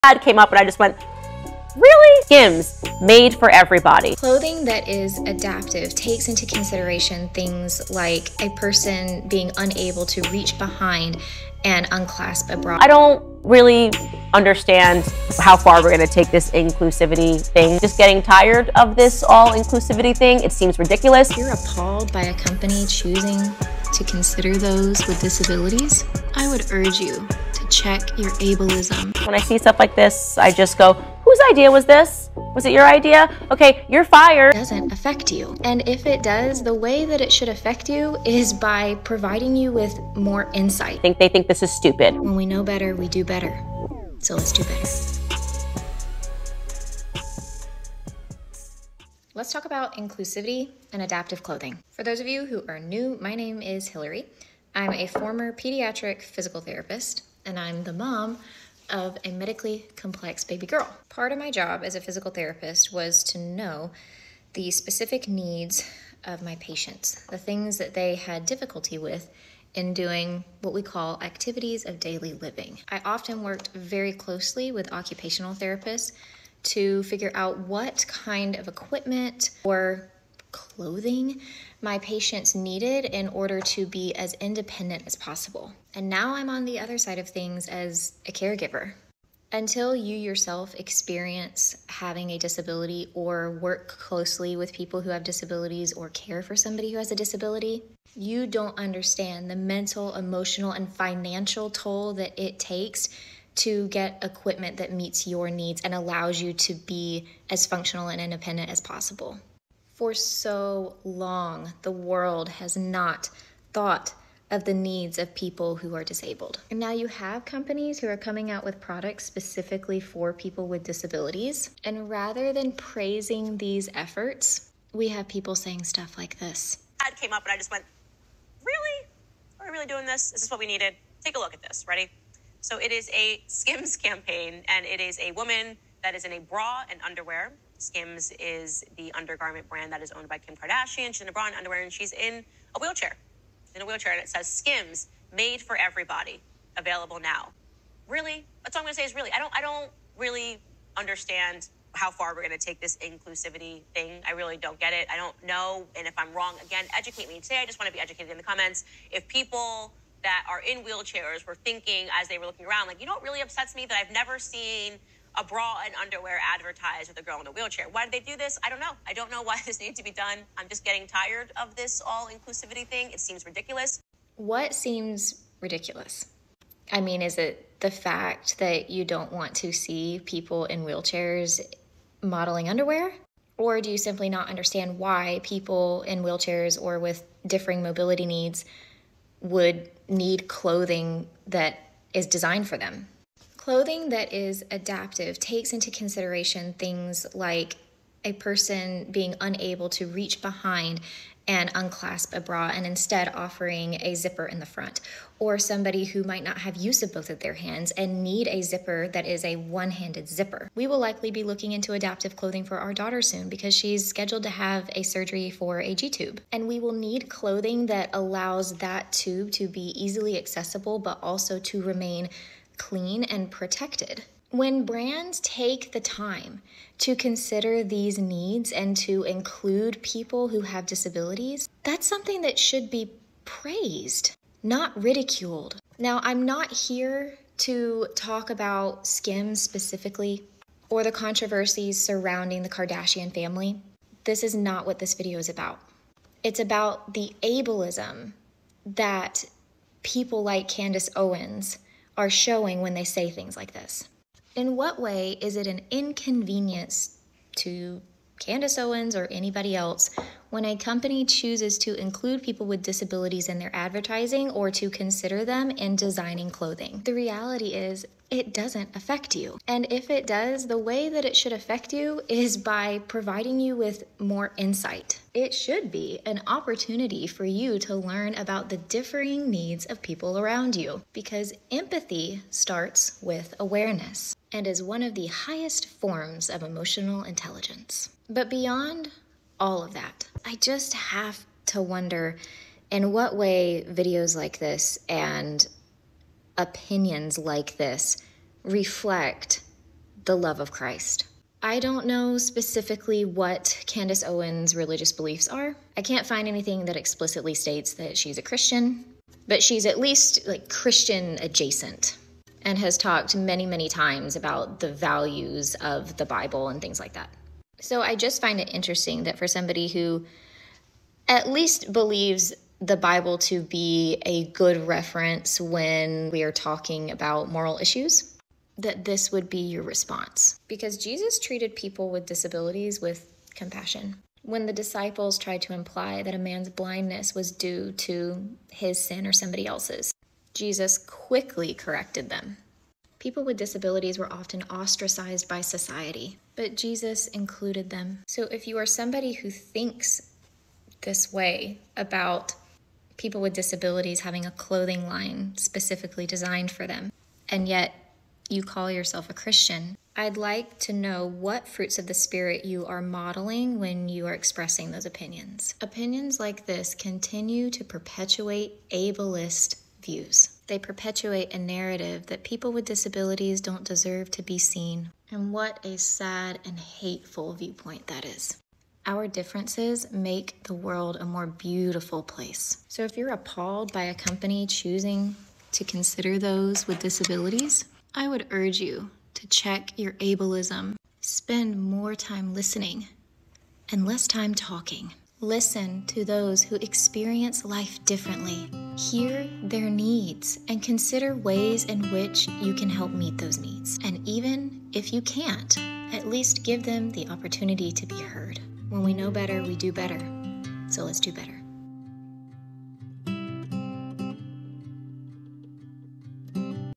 Ableism and Adaptive Clothing: A Response to Candace Owens
@channelname456  @DailyWirePlus  We should CELEBRATE each other’s differences and the companies who acknowledge them. 💜